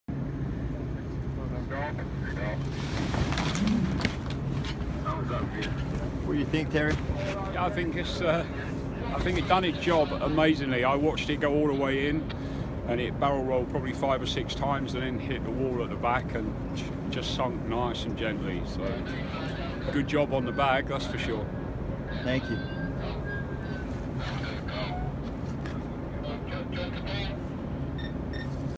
Stuntdriver Terry Grant reaction after the first robot jump into the airbag
Enter the World of BAGJUMP!